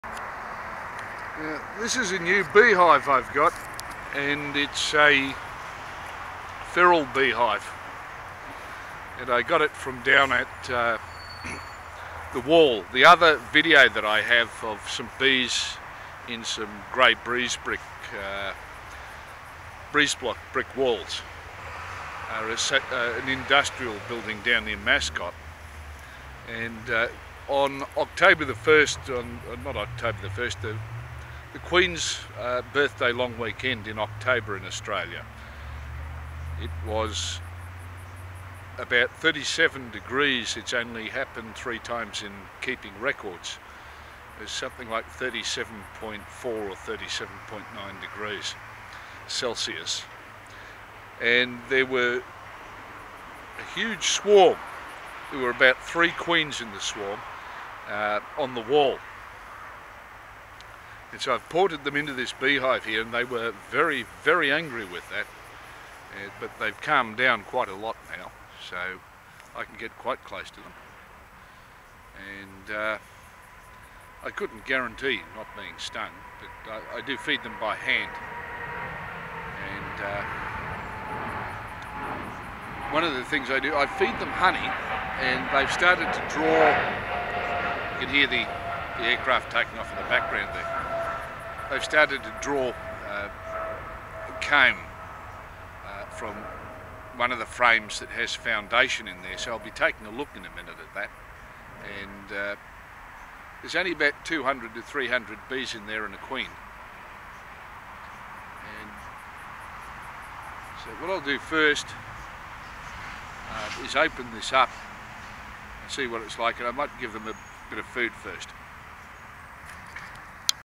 Short About - the Sydney Industrial Feral Bee Swarm Captured
[ In NSW October long weekend is the "Labour Day"(eight hour day) long weekend ]
See the hive entrance they came from in a Sydney Industrial building wall beside a footpath
(note: the feral hive entrance video shows the Langstroth hive in late mid winter with no NSW registration painted on the front - the bees were not tempted and were eventually scraped from the wall during swarm in October)
This is nine days later from the Monday ending of the Queens Birthday long weekend in October 2015
This is a swarm of feral bees scraped from a Sydney industrial building wall. They swarmed on the Queens Birthday Holiday long-weekend in October 2015. They were placed in the hive on the Monday ending.
This is about them after 9 days introduction to the hive.
The hive is a 10-full-deep Langstroth with migratory lid(will add the other sections when they are properly established).
Only one frame has foundation wax template, the others i am hoping they will draw on themselves.